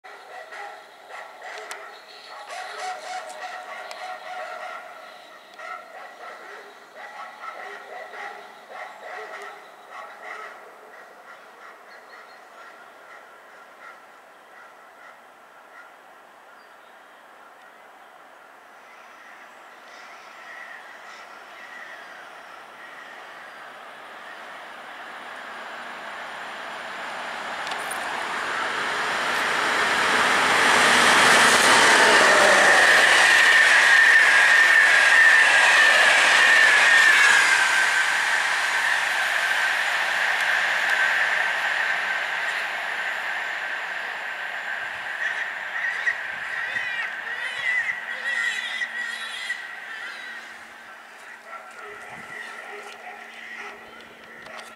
Opóźniony EIC 43 w duecie :)
Przelot EP09-034 z EU44-007 i EIC 43 do Warszawy. Data nagrania: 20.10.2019